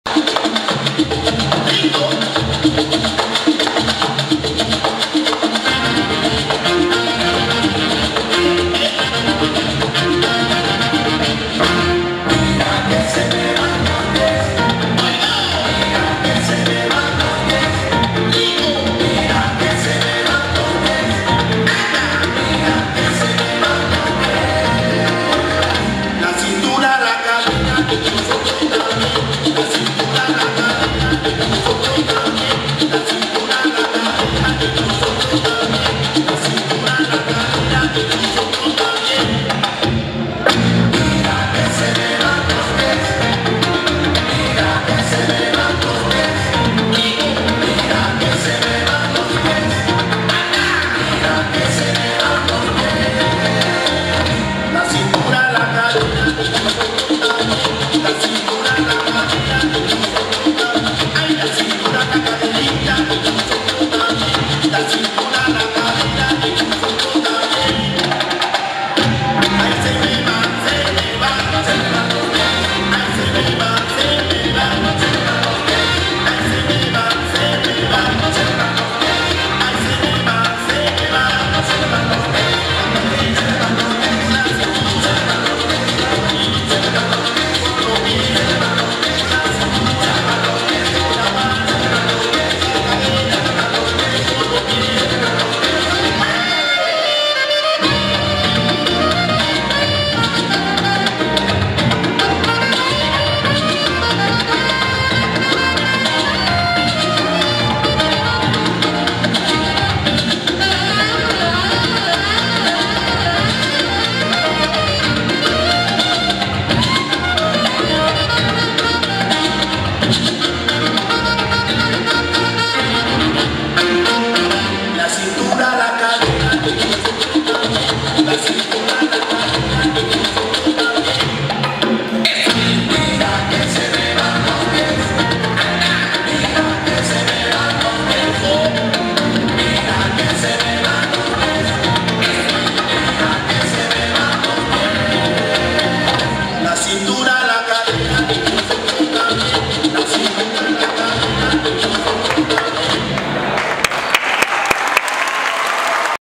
ACADEMIA "NORMA BRITO" CLAUSURA VERANO 2016
Danzas Negras. Niñas 5 a 8 años. Alumnas del Prof. José Santos Salazar. ( Lunes - Miércoles 4 a 5pm). No. 76 del Programa. Clausura auspiciada por el Ministerio de Cultura. Domingo 06 de Marzo del 2016. Teatro Municipal de Santa Anita.